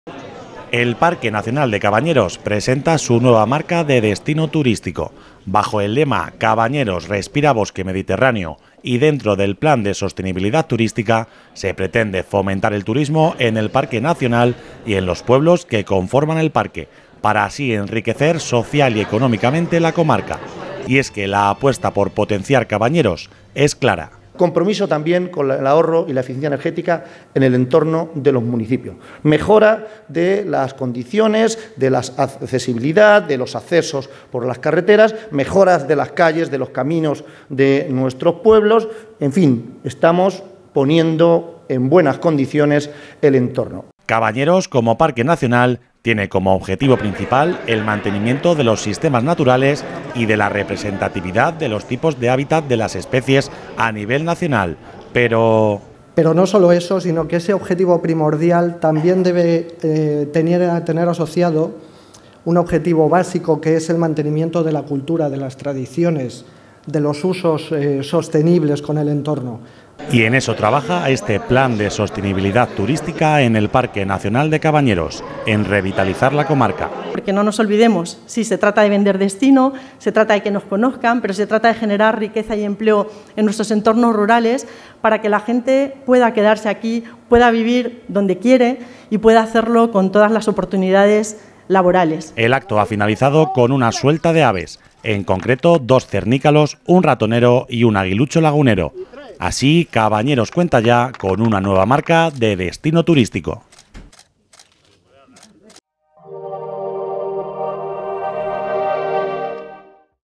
El Parque Nacional de Cabañeros presenta su nueva marca de destino turístico
El Parque Nacional de Cabañeros presenta su nueva marca de destino turístico. Bajo el lema ‘Cabañeros, respira bosque mediterráneo’ y dentro del Plan de Sostenibilidad Turística, se pretende fomentar el turismo en el Parque Nacional y en los pueblos que conforman el parque, para así enriquecer social y económicamente la comarca. Y es que la apuesta por potenciar Cabañeros es clara.